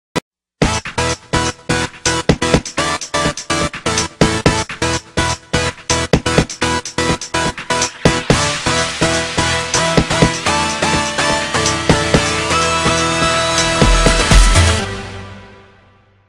Black roses. The boys band. [fictif]
Après le Girls group je vous présente le Boys band ! Créé par Shany (entertainment xD) :)

Park Bae: Leader
Lee Jae-Sun: Rappeur principal
Kim Daeho: Danseur principal
Park Hyun-Shik: Chanteur principal
Oh Ji-Seok: Maknae